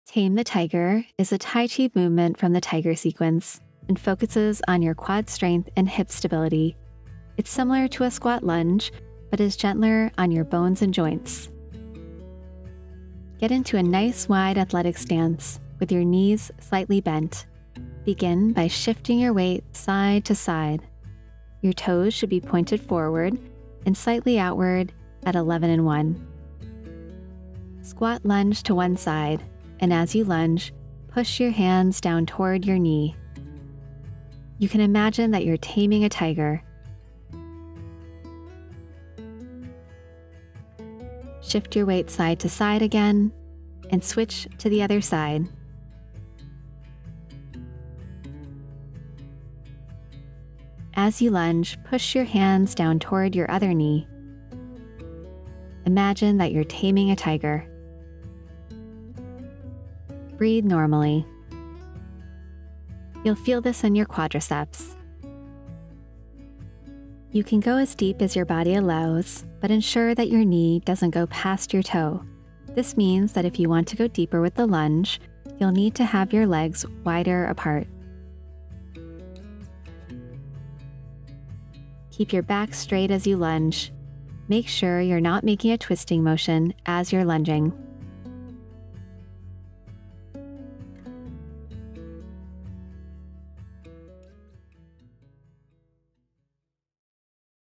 How To Properly Do Tame the Tiger (Tai Chi) - Strength & Balance for Osteoporosis - Wellen
Tame the Tiger is a tai chi movement from the tiger sequence and focuses on your quad strength and hip stability. It’s similar to a squat lunge but is gentler on your bones and joints.

⏱️TIMESTAMPS⏱️
0:00 Why do a Tame the Tiger
0:13 How to set up for a Tame the Tiger
0:17 How to perform a Tame the Tiger
1:07 Common mistakes when performing a Tame the Tiger

SET UP
Get into a nice wide athletic stance with your knees slightly bent. Begin by shifting your weight side to side. Your toes should be pointed forward and slightly outward at 11 and 1.

MOVEMENT
Squat lunge to one side and as you lunge, push your hands down toward your right knee.

You can imagine that you’re taming a tiger.

Shift your weight side to side again and switch to your other side. As you lunge, push your hands down toward your other knee.

TIPS
- You’ll feel this in your quadriceps.
- You can go as deep as your body allows but ensure that your right knee doesn’t go past your right toe. This means that if you want to go deeper with the lunge, you’ll need to have your legs wider apart.
- Keep your back straight as you lunge. Make sure you’re not making a twisting motion as you’re lunging.